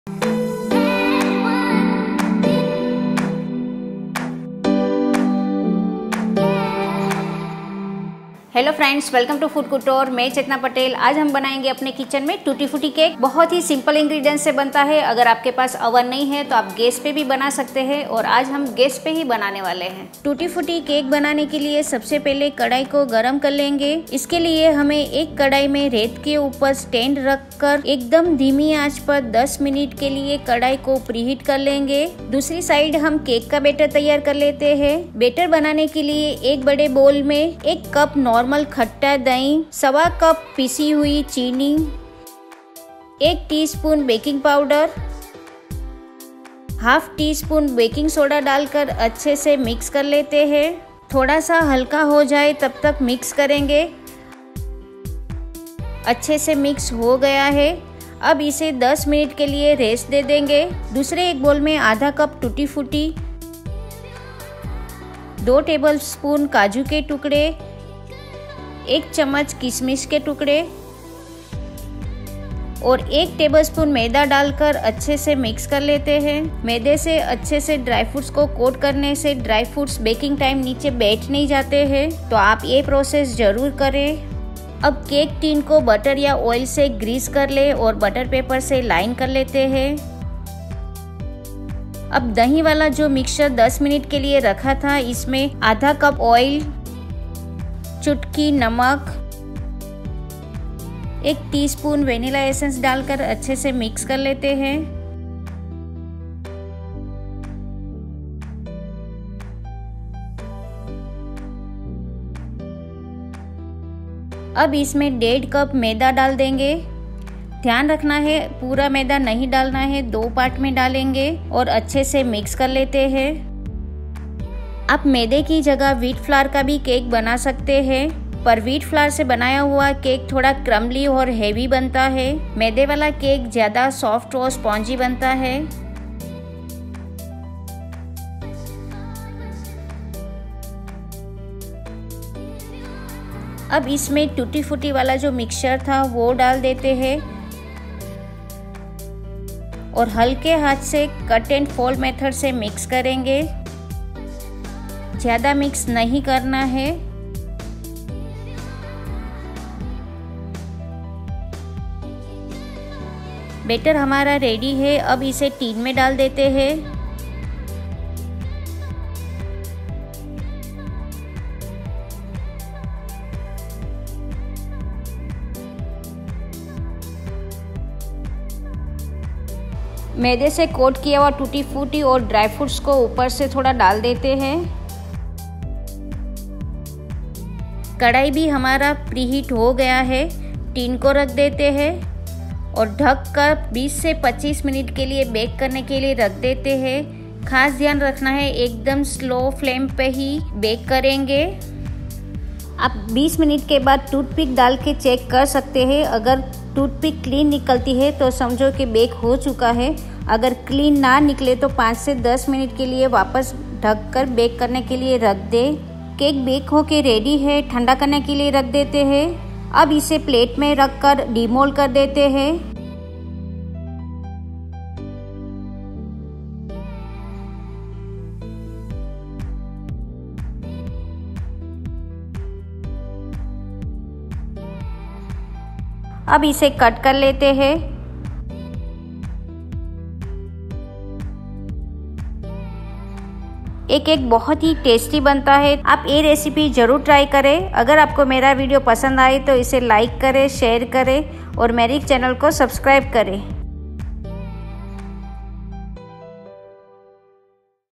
कुकर में बनाएं केक | Tutti Frutti Cake | बिना अंडे बिना ओवन के | Eggless Cake
Ingredients:

Batter :
1 cup - Curd
1+ 1/4 cup - Sugar Powder
1 tsp - Baking Powder
1/2 tsp - Baking Soda

Beat well and keep 10 min

Coating ingredients:
1/2 cup - Tutti frutti
2 tbsp - Cashew-nut Chopped
1 tbsp - Raisin
1 tbsp - Maida

After Resting 10 min put

1/2 Cup - Oil ( non flavoured)
Big Pinch - Salt
1 tsp - Vanilla essence
1 + 1/2 Cup - Maida

Put Maida in 2 Batches

Mix well and put tutti frutti coated in cut n fold method

Pour batter in loaf tin with Dusting sides

Put upper side tutti frutti and dry fruits

Baking:
Oven : preheat 10 min for 180 degree and bake for 30 -40 min till done ...
Microwave: Convection mode : preheat 10 min for 180 degree and bake 30-49 min

Check with toothpick

Slice and serve